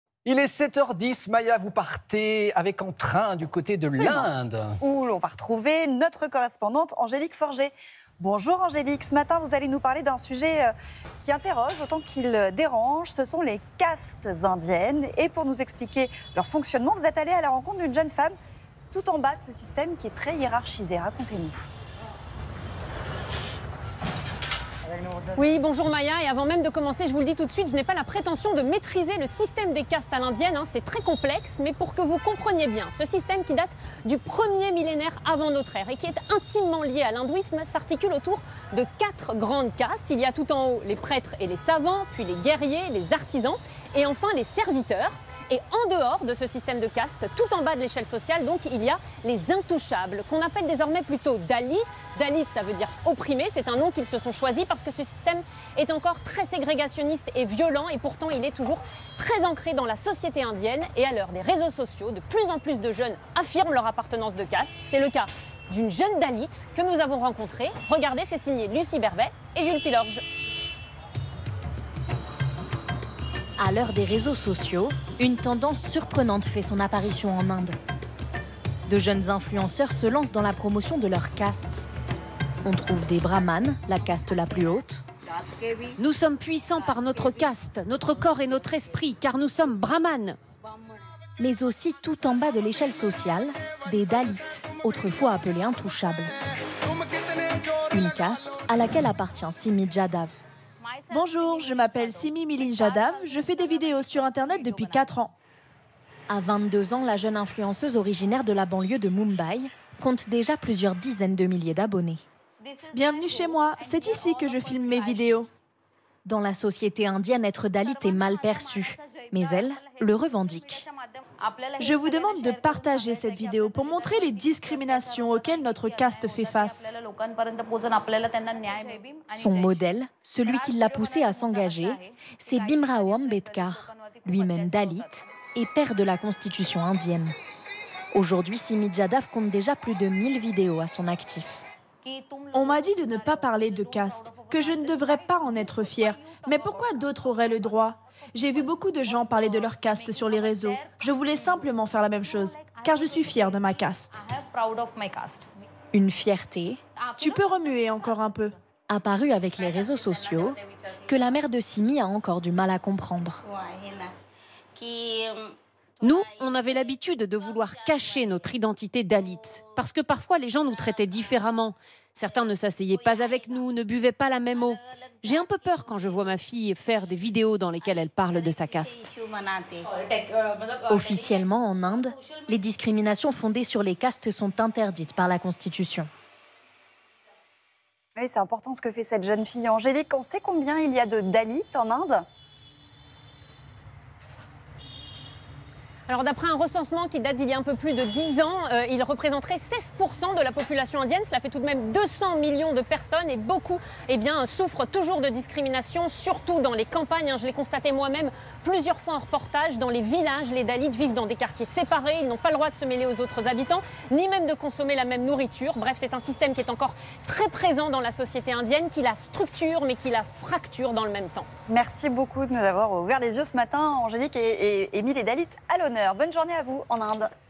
Inde : Les castes s'affirment sur les réseaux sociaux
🇮🇳 Les castes en Inde forment un système social complexe, étroitement lié à l'hindouisme. On distingue traditionnellement quatre grandes catégories : les brahmanes (les prêtres et les enseignants), les kshatriyas (les guerriers et les dirigeants), les vaishyas (les commerçants) et les shudras (les serviteurs). En dehors de ces groupes, les dalits, souvent appelés "intouchables", subissent des discriminations. Bien que ce système soit officiellement aboli, il continue d'influencer la société indienne.

Un reportage signé Angélique Forget, Lucie Berbey, et Jules Pilorge